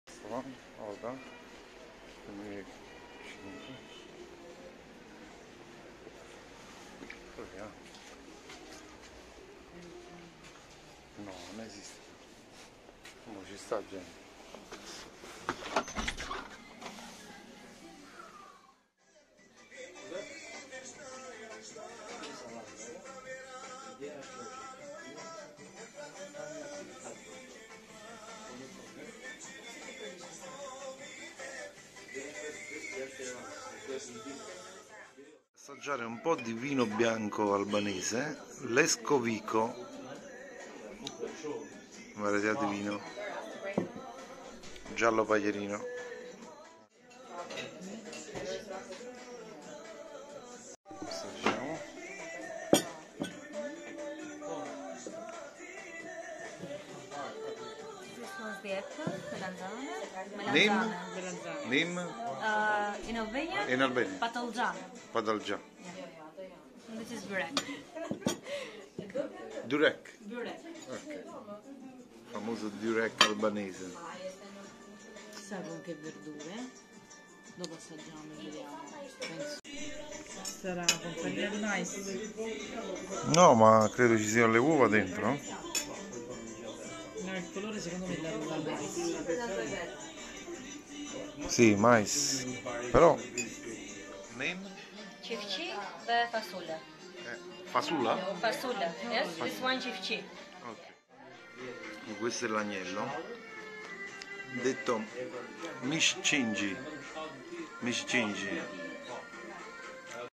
PIATTI TIPICI ALBANESI A TIRANA: RISTORANTE ODO
Questo grazioso e accogliente ristorantino si trova in una piccola traversa di Rruga Luigj Gurakuqi, Tirana, Albania. Vi consigliamo la visita.